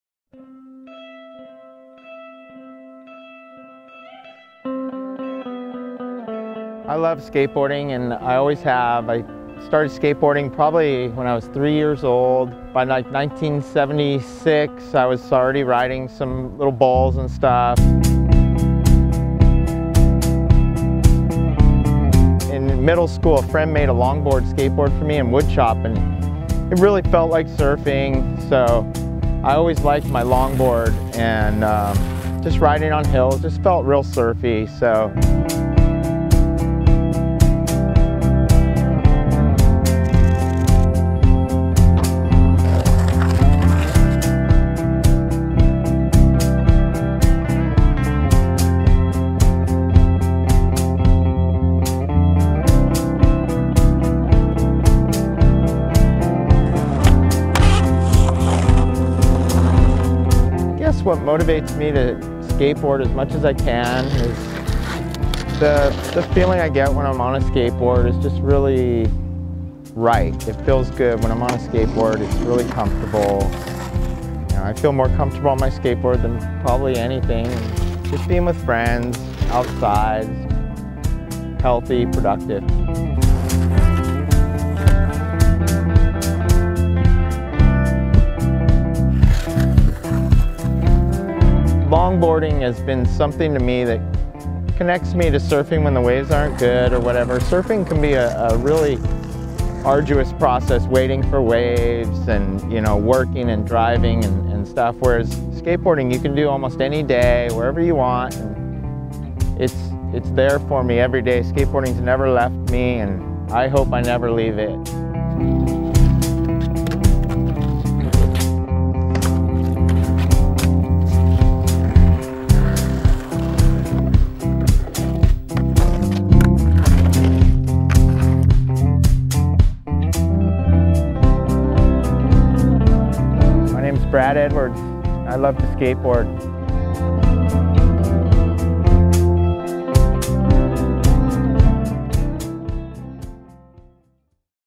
Gravity Skateboards - This Is Brad Edwards - 40" Brad Edwards & 43" Drop Kick
Brad Edwards skating some hills and ditches while filling us in on how he became the skater that he is today.